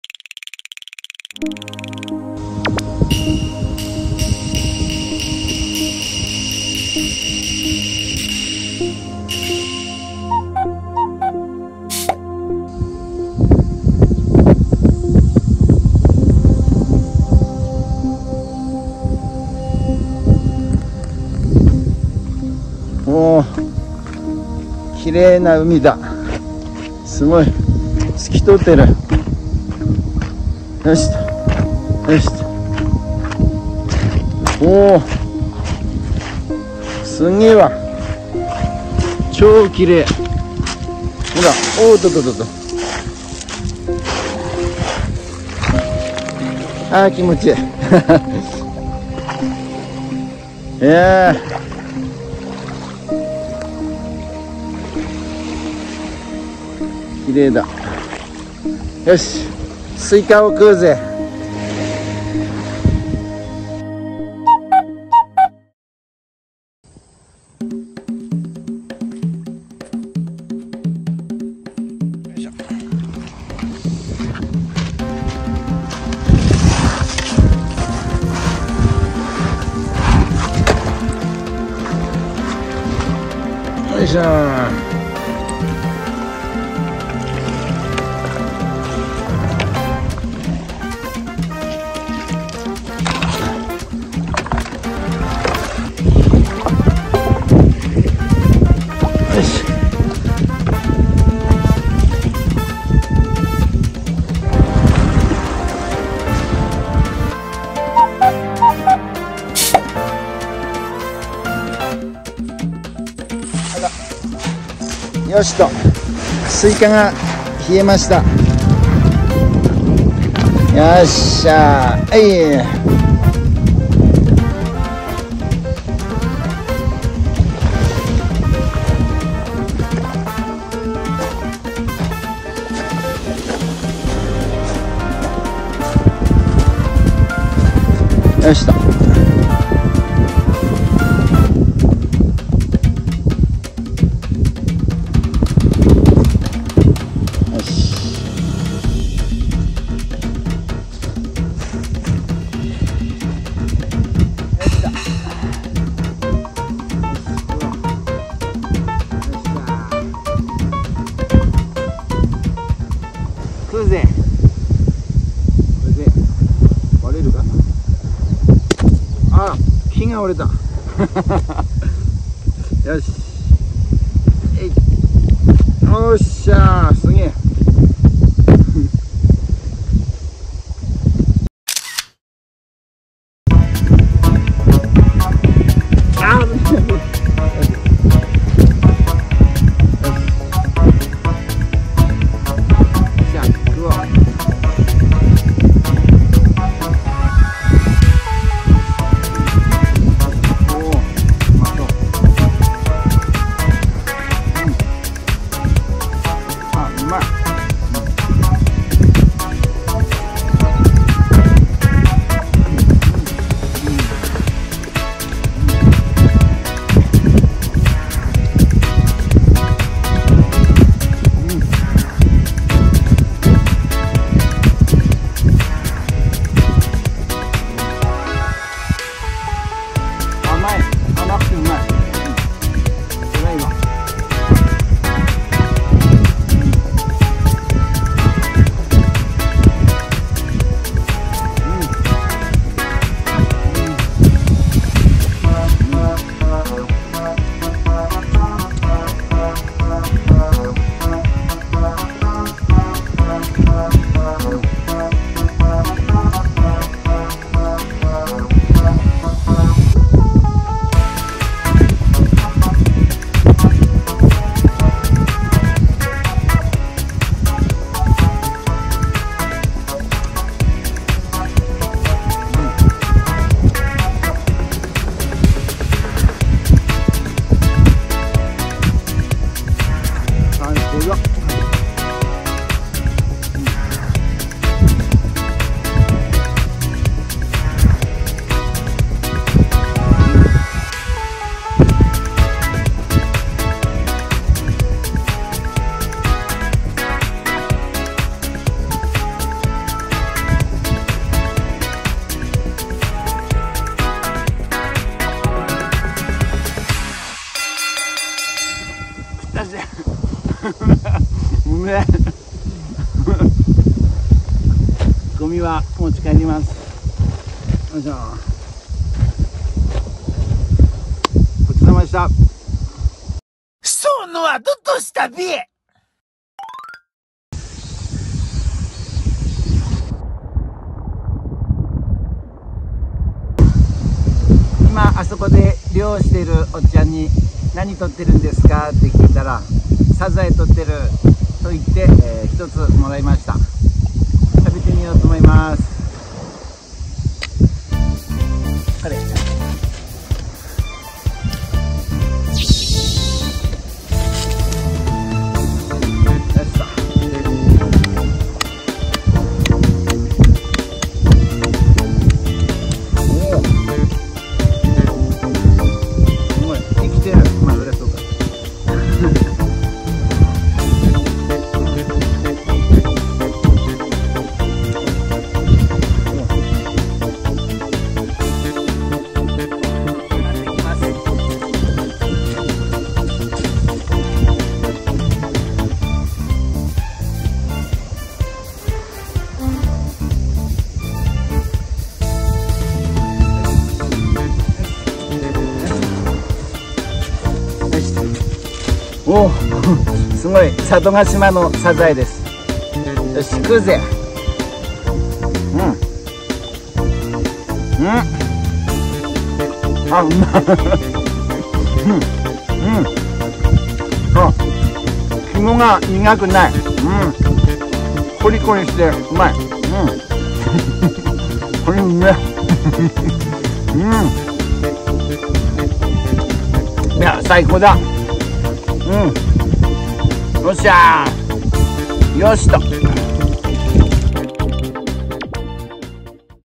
佐渡ヶ島キャンプ２です。漁をしていた漁師さんに頂いたサザエと佐渡ヶ島の八百屋さんで買った佐渡ヶ島産のスイカを豪快に食べます。
佐渡ヶ島キャンプ２です。
佐渡ヶ島キャンプ１をまだみてない方はそちらの方もごらんください。